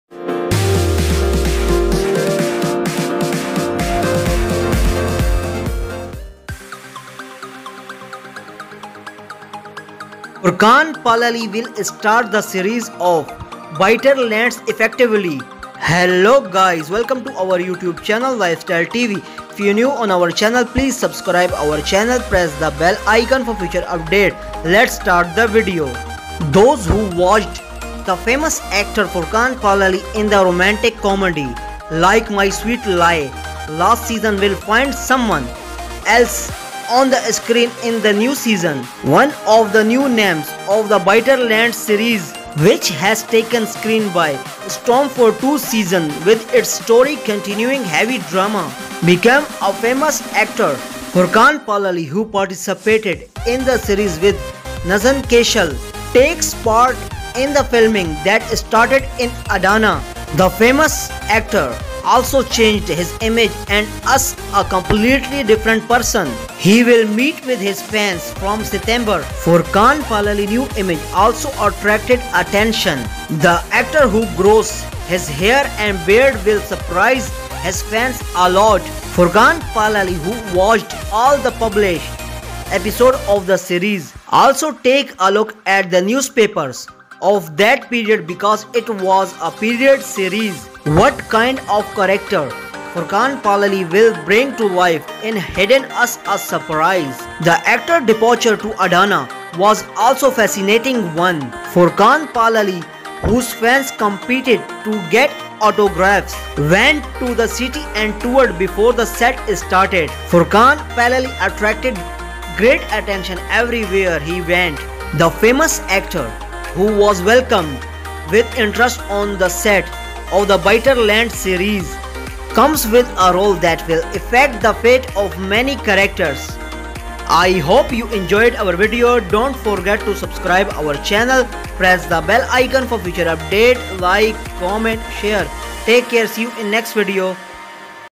Furkan Palalı Will Start The Series Of Bitter Lands Effectively
Turkish Translated